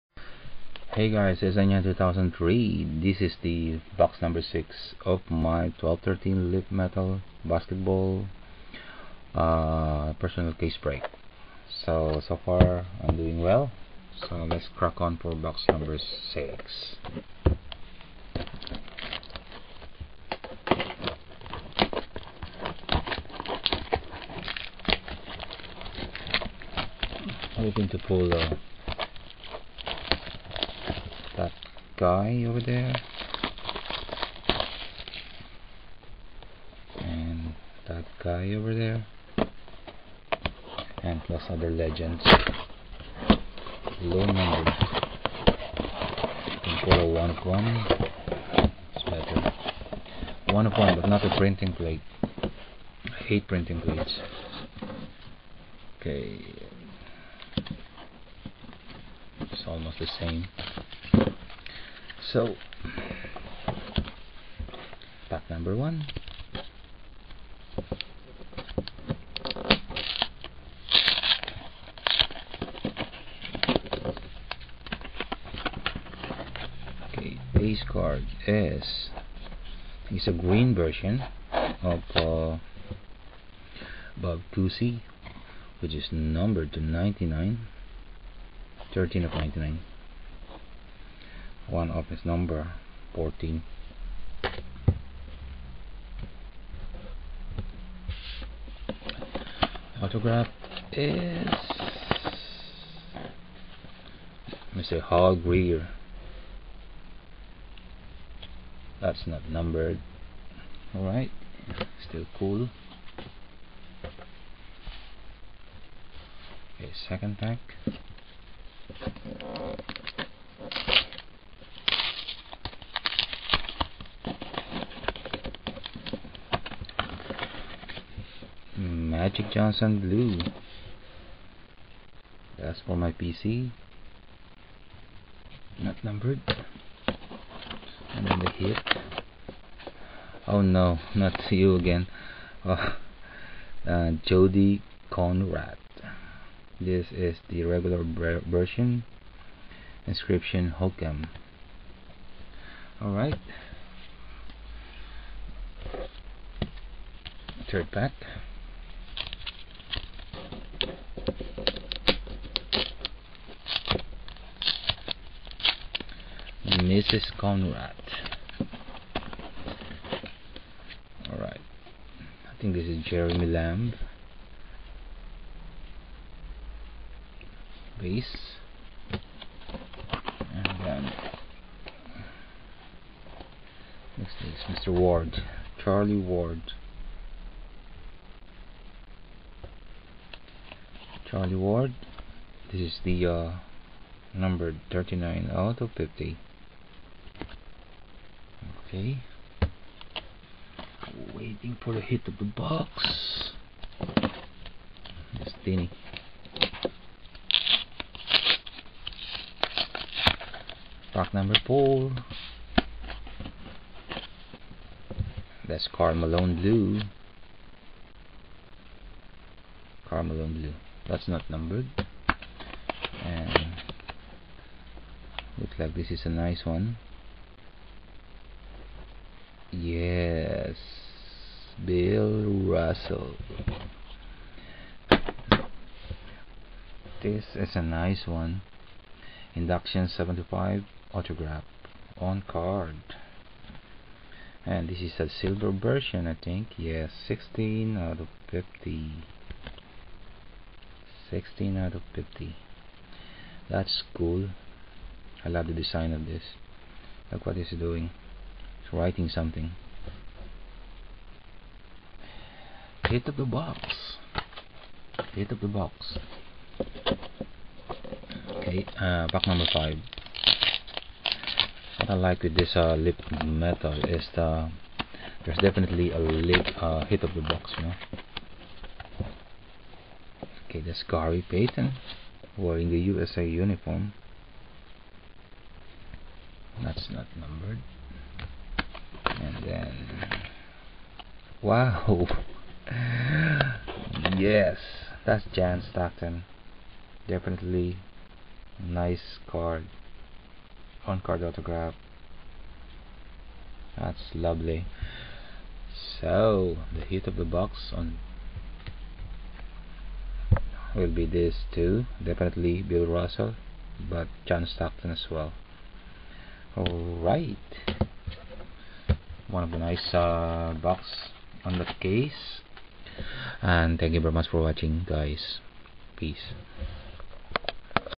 CSNYOUNG2003 - 2012-13 Leaf Metal Basketball 8 Box Case Break - Box #6
15th November 2013

My personal case of 2012-13 Leaf Metal Basketball

PM me if you;re interested on any cards that I pulled.

For the meantime, I'm selling only.